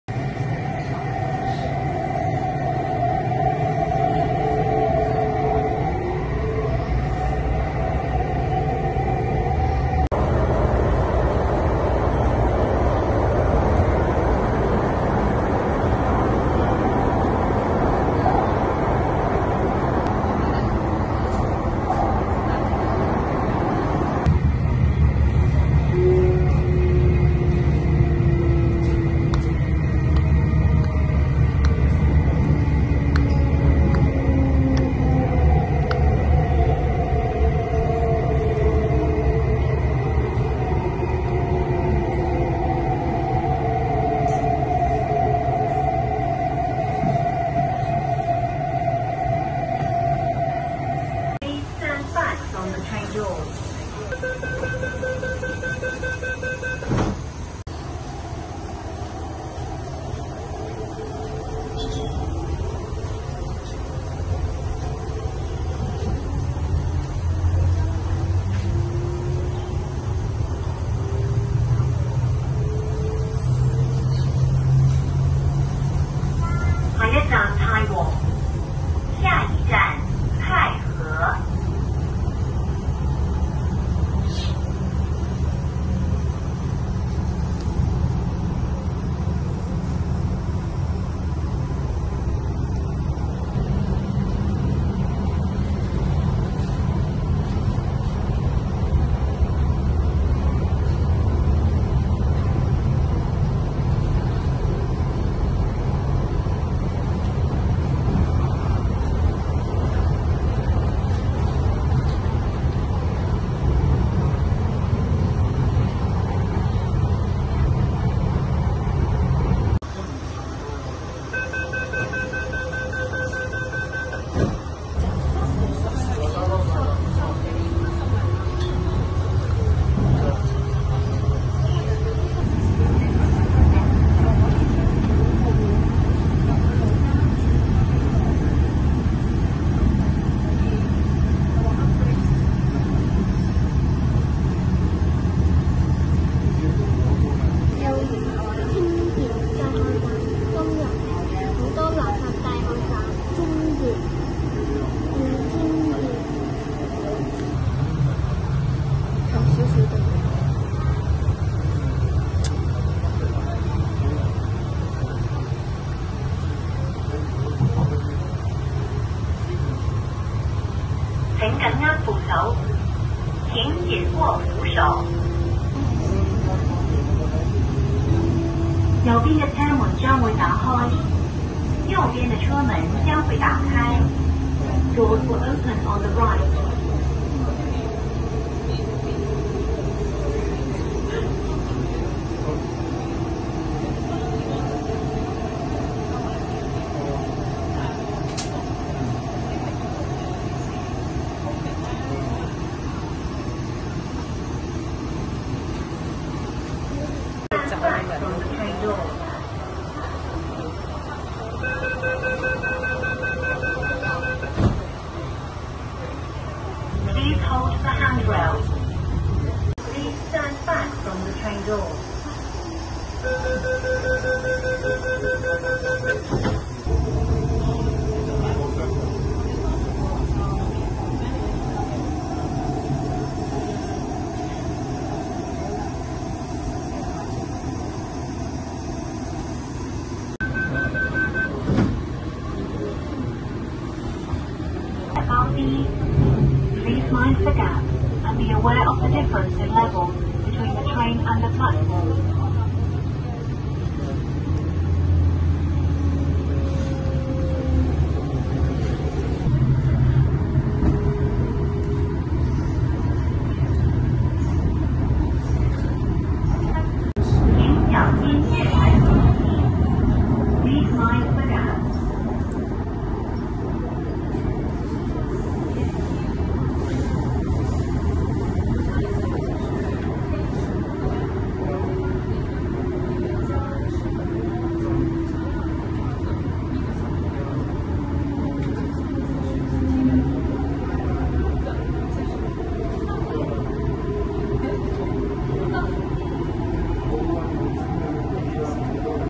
Lok Ma Chau to Hum Hom via Racecourse 落馬洲經馬場往紅磡
This video is talking about Lok Ma Chau to Hum Hom via Racecourse
If you want to go to Fo Tan please interchange in University.